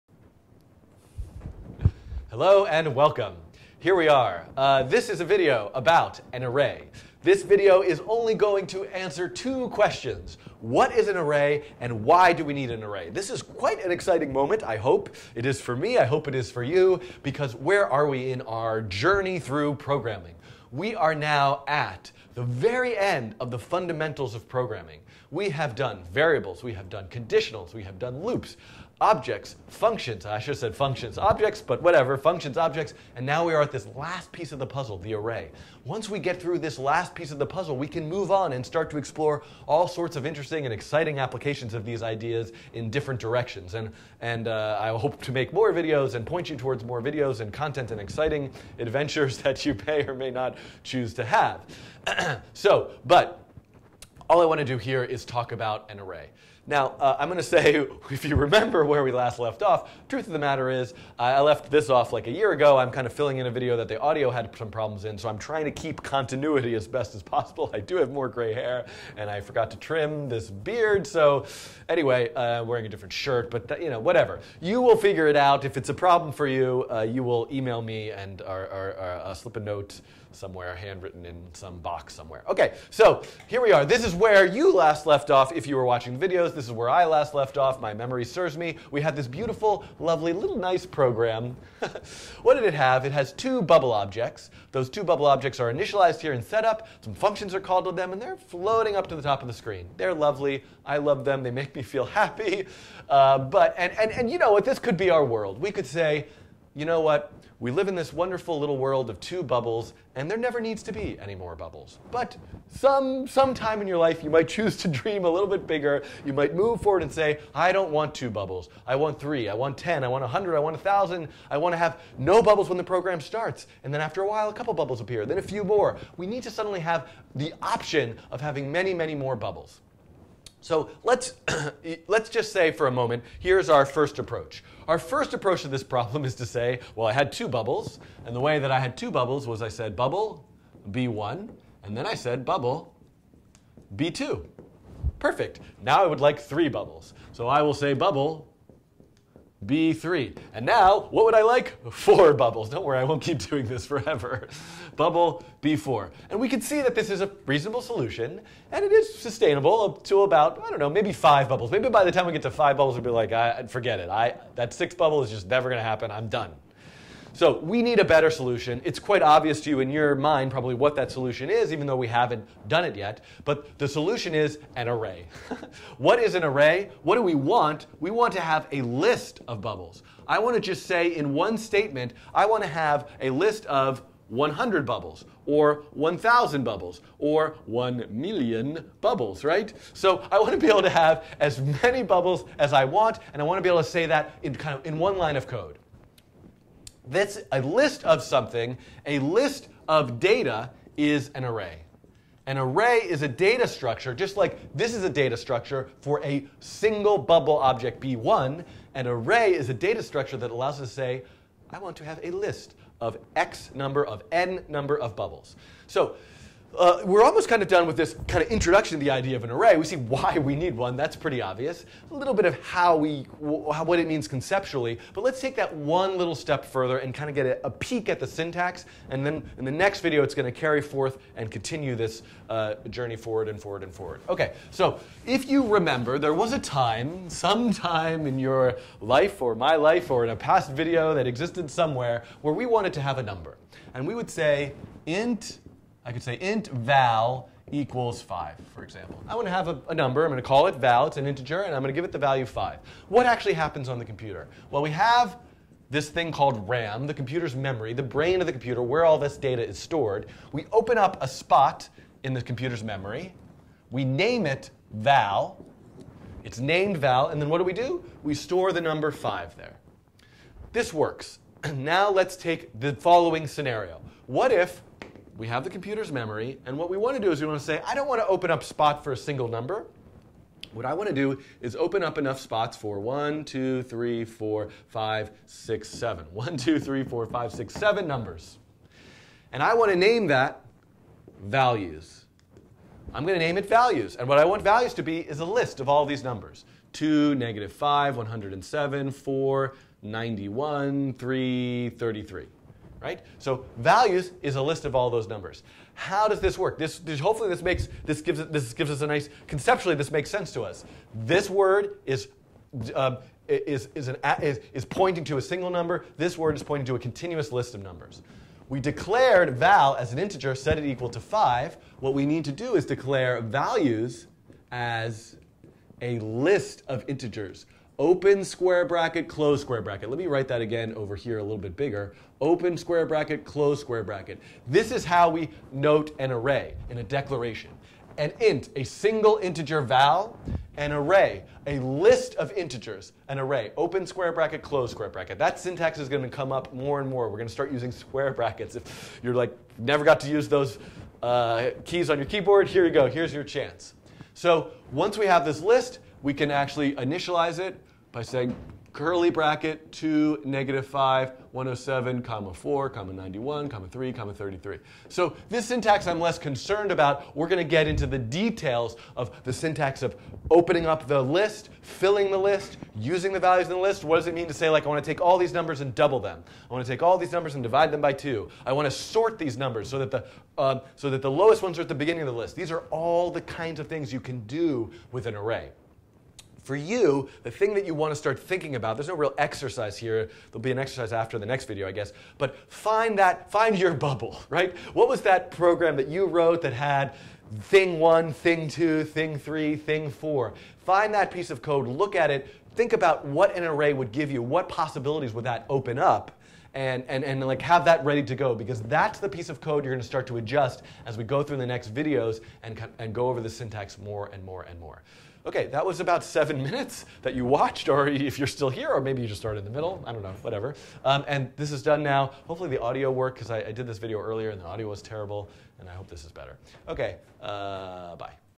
9.1: What is an Array? - Processing Tutorial
This covers looks at the concept of an array and why we need them.

Book: Learning Processing A Beginner's Guide to Programming, Images,Animation, and Interaction

Chapter: 9